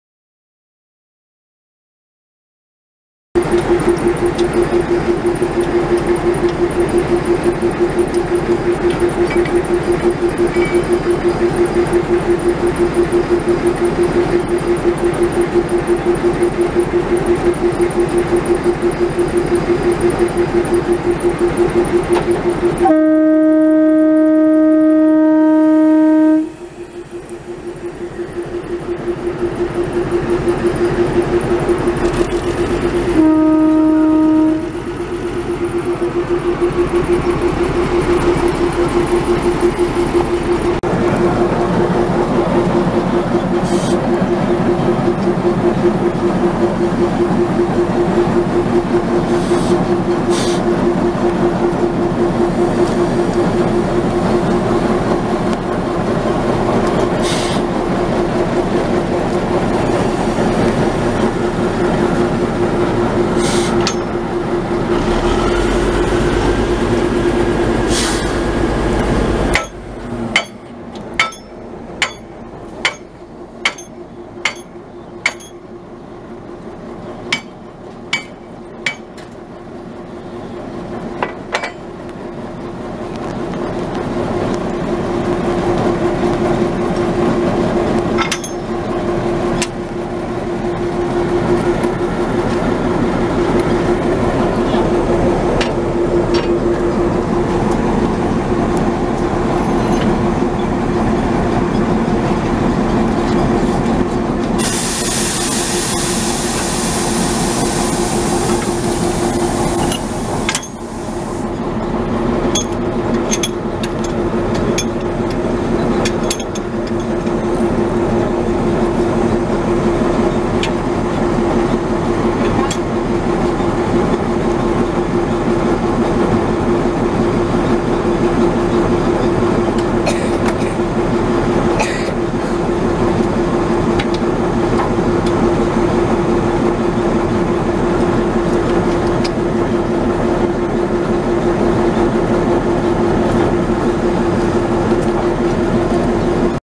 Coupling of Narrow gauge train | Decoupling and coupling of Narrow gauge train Loco | Rare video
Decoupling and coupling of Narrow gauge train locomotive at Waghai ( Bilimora-Waghai stretch) Western Railways.